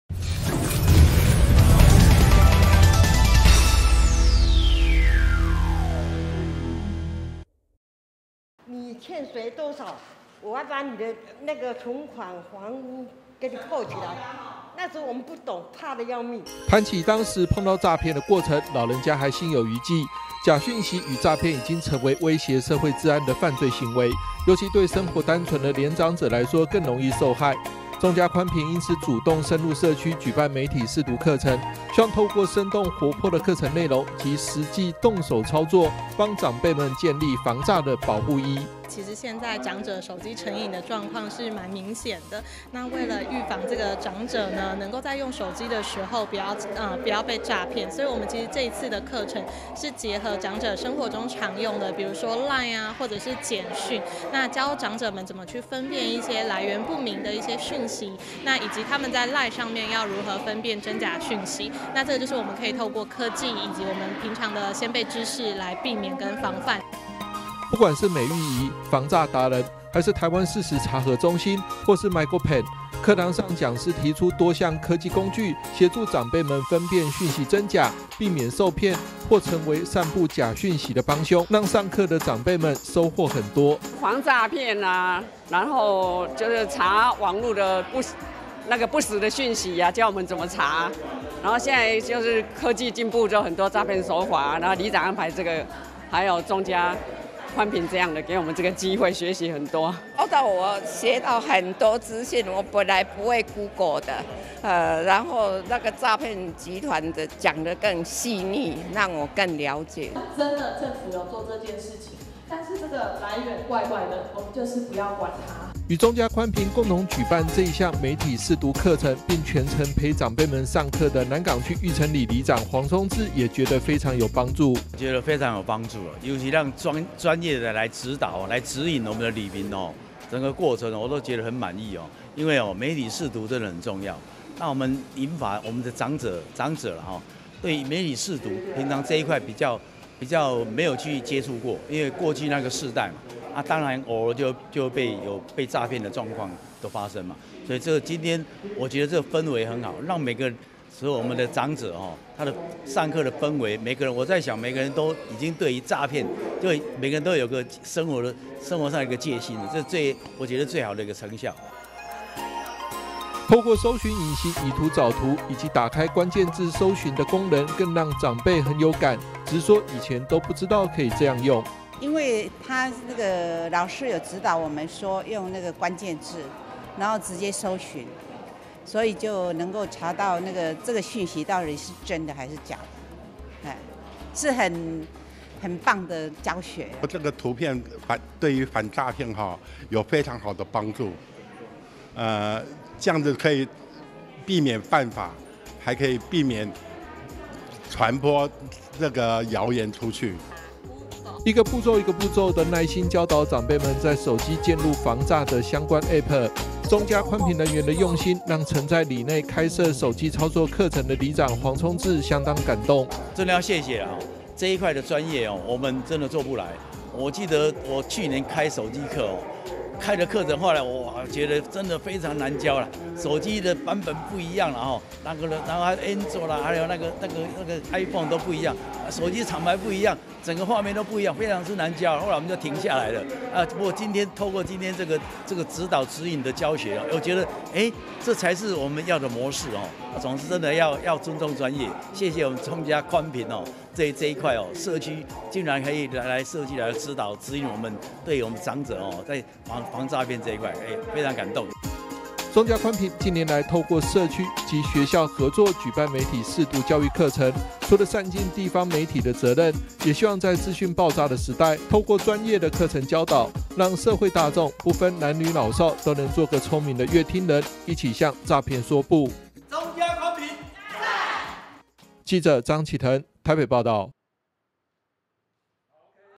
詐騙 假訊息|銀髮 長者|防詐 打假|手機 AI|臉書 Line|媒體 識讀|2023 中嘉新聞|向詐騙及假訊息說不 中嘉寬頻深入社區宣導防詐 幫助長輩打假
助長輩向詐騙說不 中嘉寬頻講師深入社區教銀髮族防詐絕招 科技防詐最有效

（中嘉新聞記者張啓騰/台北南港區報導）詐騙案件頻傳，影響社會治安甚鉅，除了靠警方積極查緝外，也需要國人提高警覺小心防範，中嘉寬頻因此深入社區結合當地里長舉辦媒體識讀課程教導民眾如何防詐，尤其結合年長者生活中常用的通訊軟體，教長輩們如何透過手機科技分辨訊息真假，避免受騙，更不當假訊息的傳播者。

談起當時碰到詐騙的過程，老人家還心有餘悸，假訊息與詐騙已經成為威脅社會治安的犯罪行為，尤其對生活單純的年長者來說更容易受害，中嘉寬頻因此主動深入社區舉辦媒體識讀課程，希望透過生動活潑的課程內容及實際動手操作幫長輩們建立防詐的保護衣。

不管是美玉姨、防詐達人，還是台灣事實查核中心或是MYGOPEN，課堂上講師提出多項科技工具協助長輩們分辨訊息真假避免受騙或成為散佈假訊息的幫兇，讓上課的長輩們收穫很多。

與中嘉寬頻共同舉辦這場媒體識讀課程並全程陪長輩們上課的南港區玉成里長黃聰智也覺得非常有幫助。

透過搜尋引擎以圖找圖以及打關鍵字搜尋的功能更讓長輩很有感，直說以前都不知道可以這樣用。

一個步驟一個步驟地耐心教導長輩們在手機建立防詐的相關APP，中嘉寬頻人員的用心讓曾在里內開設手機操作課程的里長黃聰智相當感動。

中嘉寬頻近年來透過與社區及學校合作舉辦媒體識讀教育課程，除了善盡地方媒體的責任，也希望在資訊爆炸的時代，透過專業的課程教導讓社會大眾不分男女老少都能做個聰明的閱聽人，一起向詐騙說不。